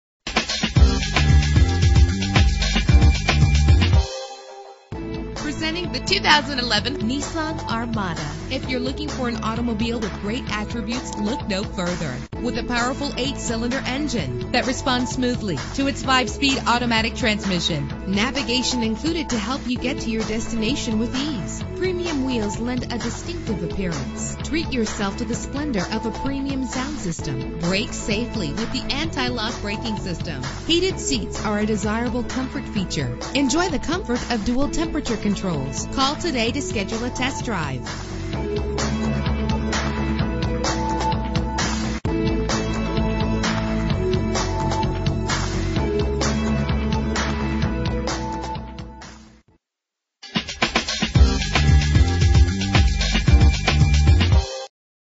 2011 Nissan Armada - Philadelphia PA
Year: 2011
Make: Nissan
Model: Armada
Trim: Platinum
Engine: 5.6 liter 8 cylinder 32 valve
Transmission: 5-Speed Automatic
Color: Tuscan Sun
Mileage: 0
Address:
6723 Essington Ave.